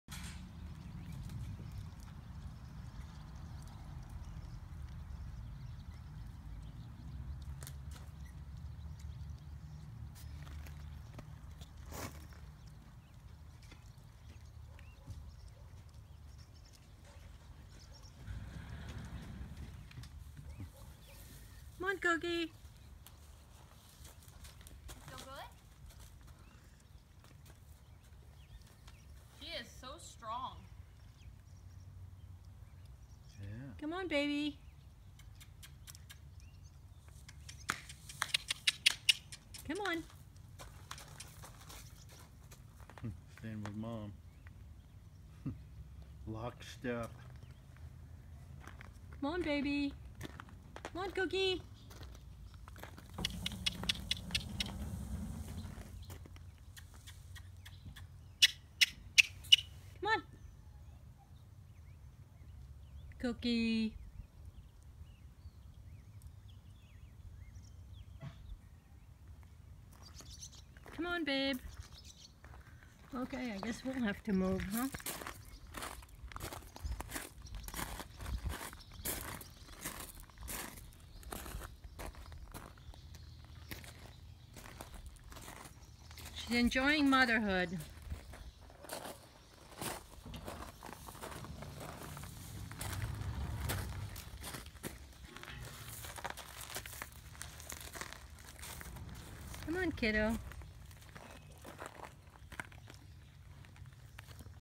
Adelina NW and Newborn Filly by Banderas
May 13th 2016. Cookie's first baby!